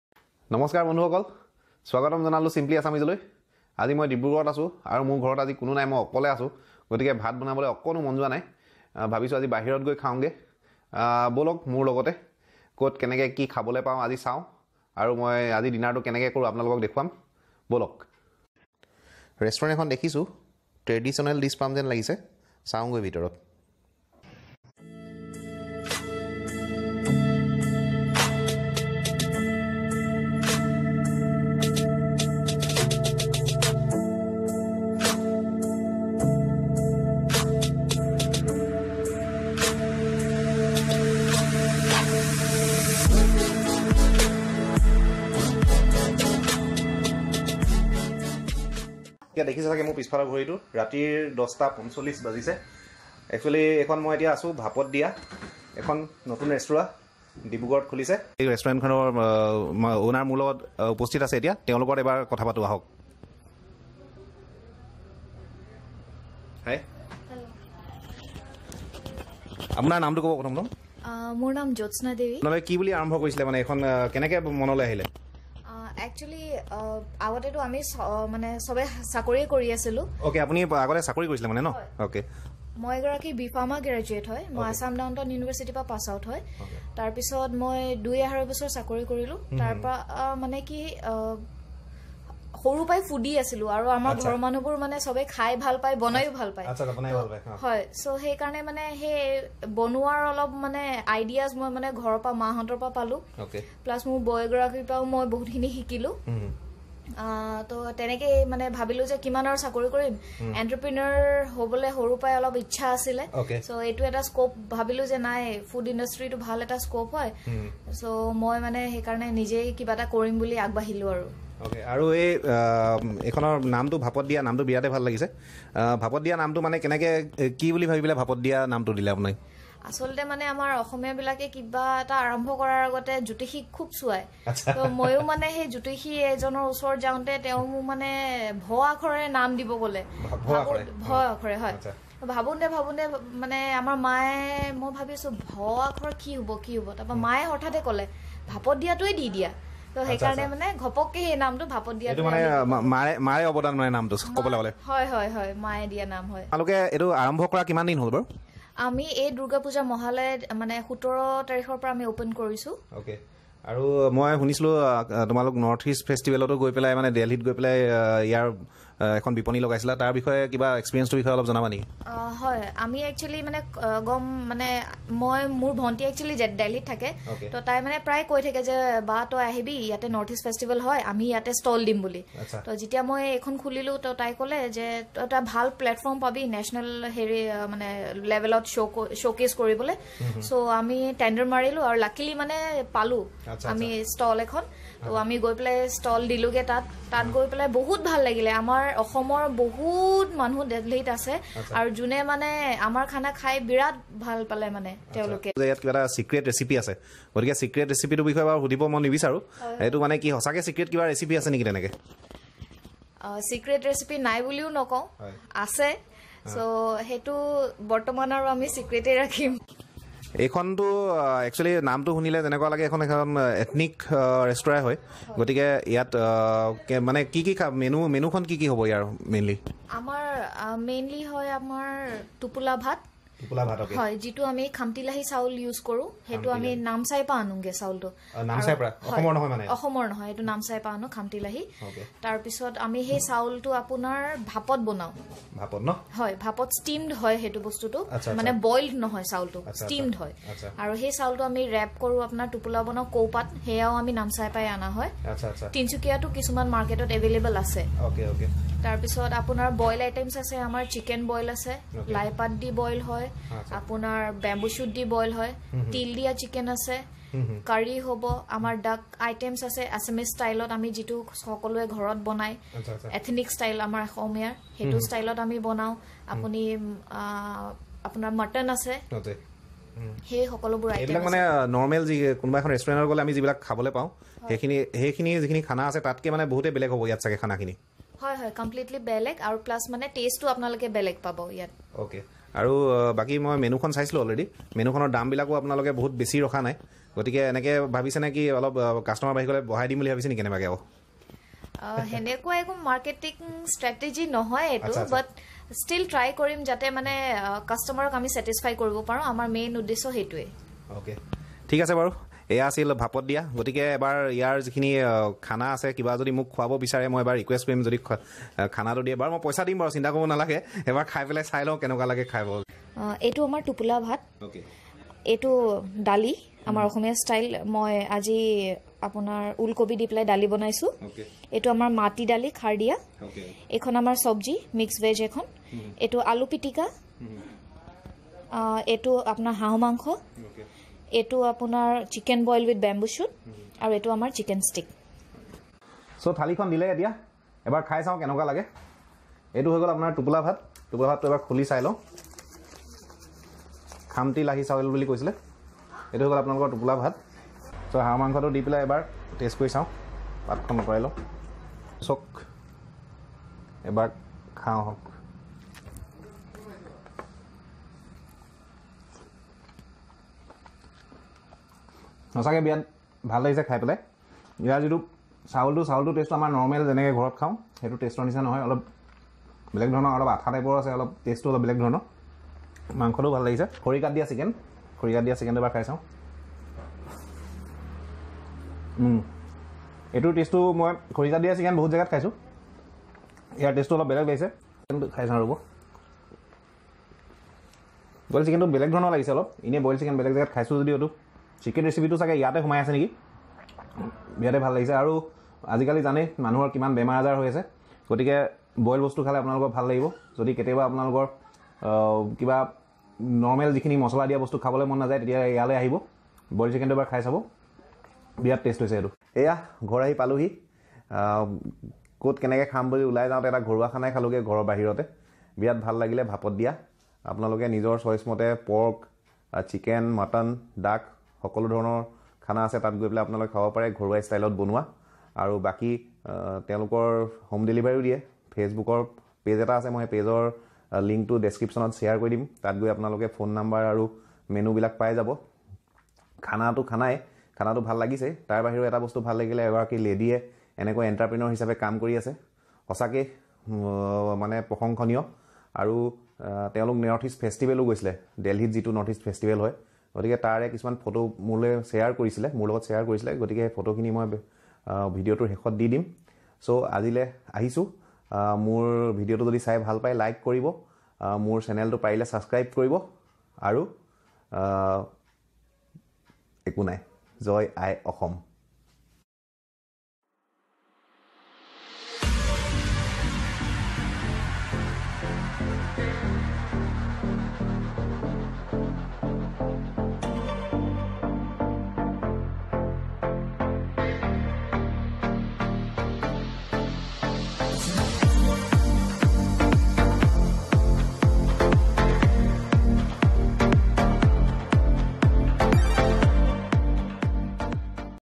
Entrepreneur ep. 2 - Dinner at Bhapot Diya, an ethnic restaurant - Dibrugarh - Simply Assamese
Address - ASTC campus, Chowkidinghee, Dibrugarh Assam